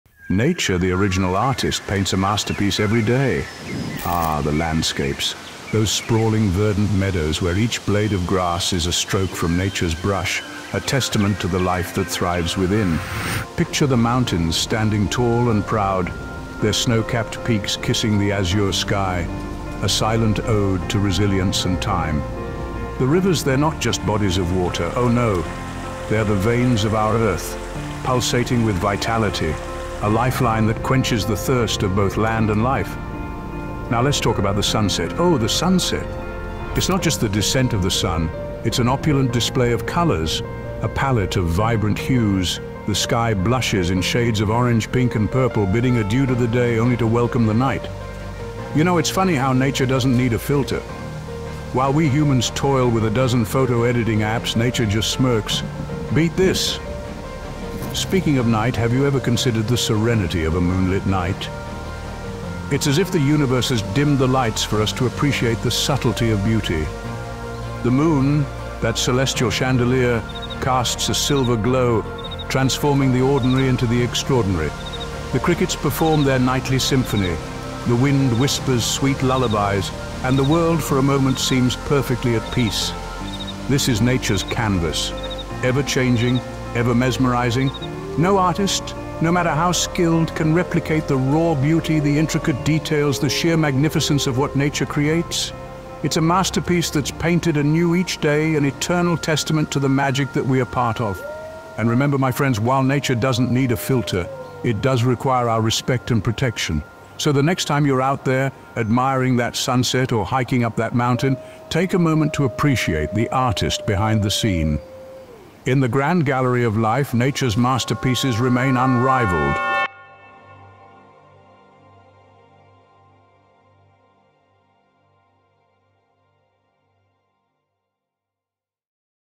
Nature, The Original Artist - Cinematic Short Film - 4K
Nature, the original artist, paints masterpieces across the canvas of the Earth. From sunrise hues that gently kiss the sky to the intricate patterns etched in the bark of ancient trees, nature's creativity knows no bounds. The rhythmic dance of waves, the symphony of birdsong, and the delicate bloom of flowers all testify to a divine artistry that predates human existence. Mountains stand as majestic sculptures, and rivers carve intricate sculptures through landscapes. In every leaf, every petal, and every gust of wind, nature whispers its timeless poetry. Its brushstroke is the embodiment of harmony, reminding us that the truest beauty lies in the unspoiled creations of the original artist.